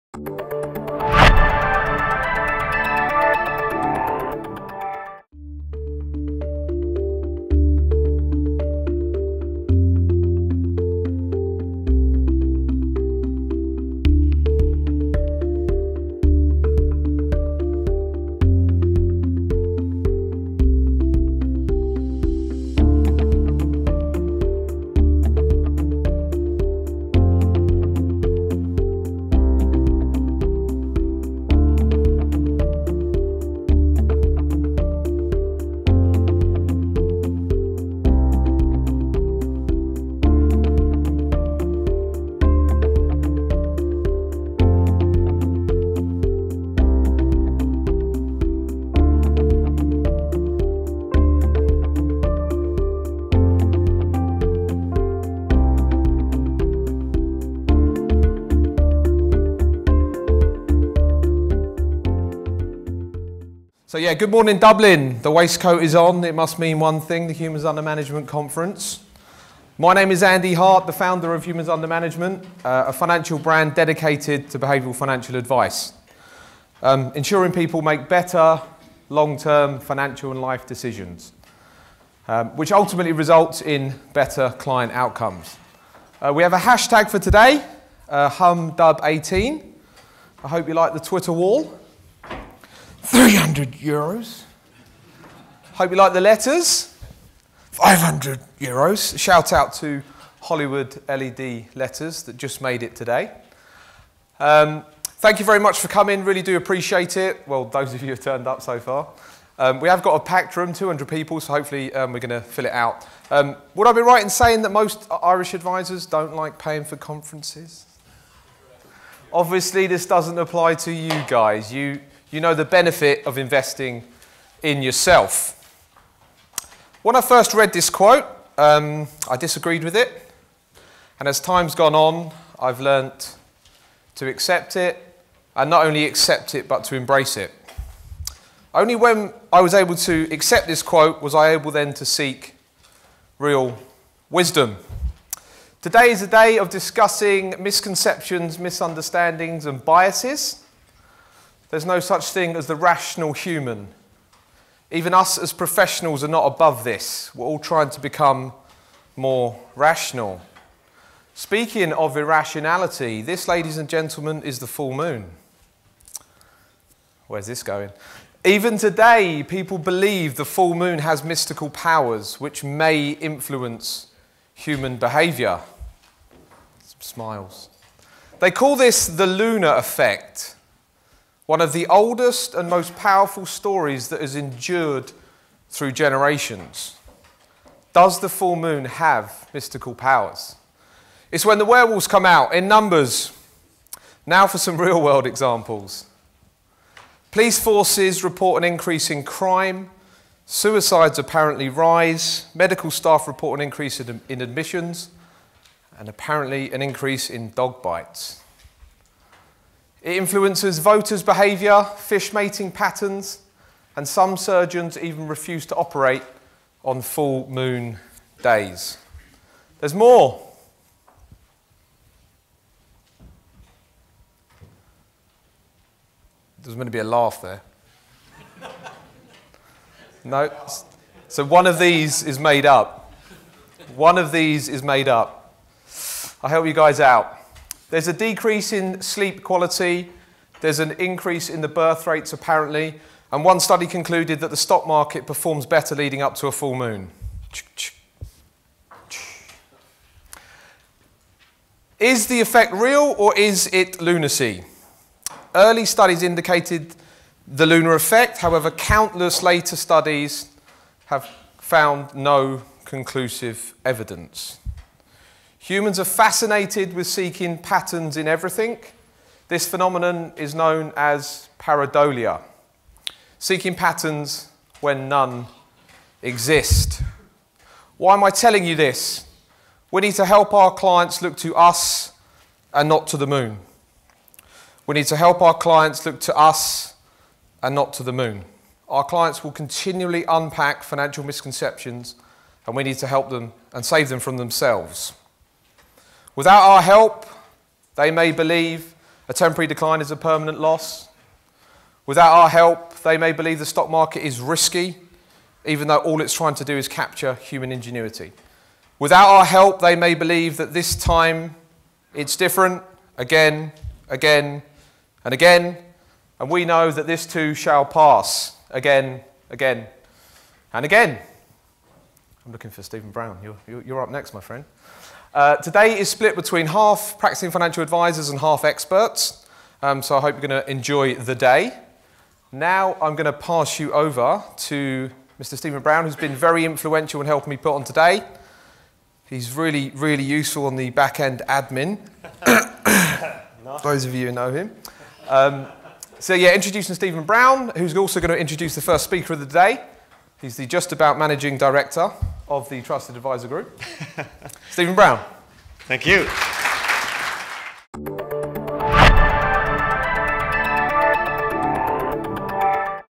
Humans Under Management - Dublin 2018
Behavioural Science for Financial Advisors Seminar in UCD Dublin.